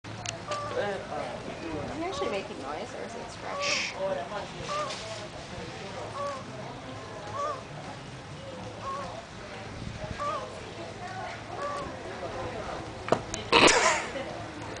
turtle humping
two turtles humping, filmed on my trip to south america (pg-13)